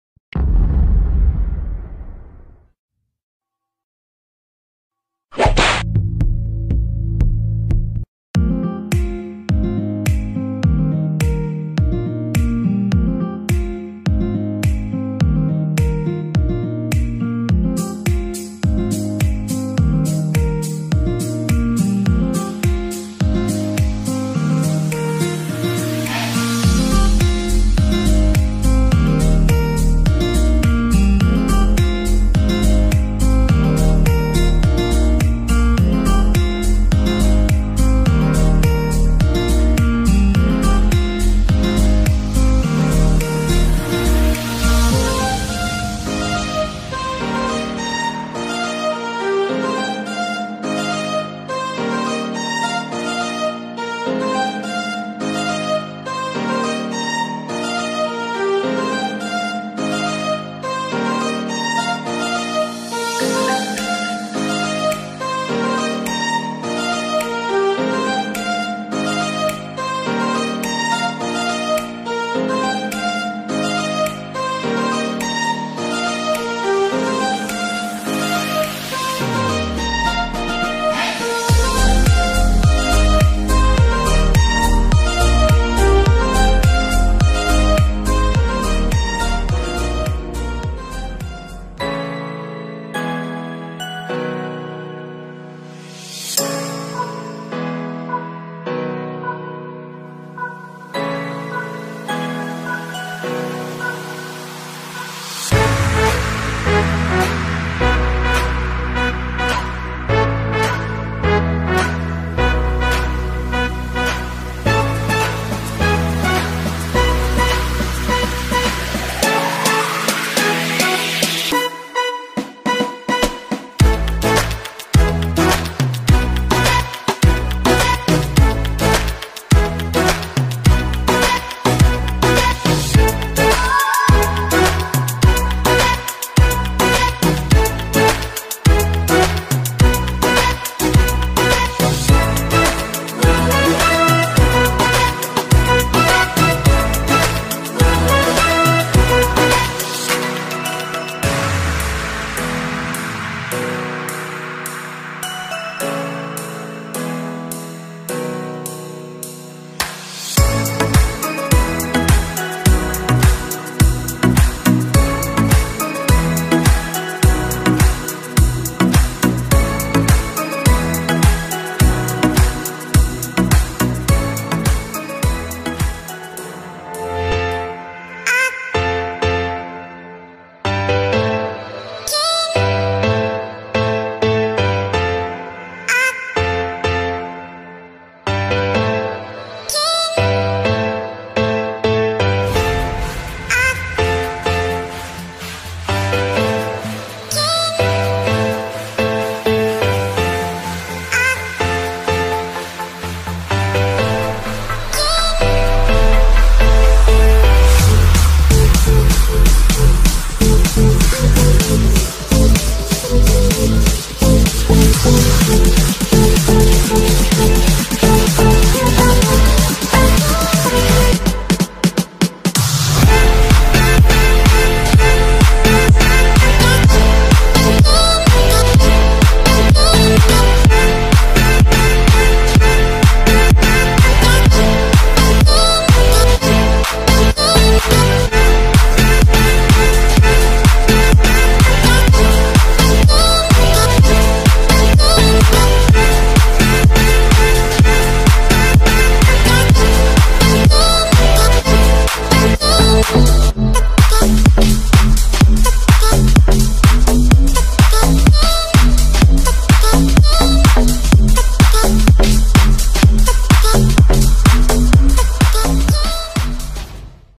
Einstein School for kids TEAM ARCHIMEDES 2018
Highlights during our family day last December 16, 2018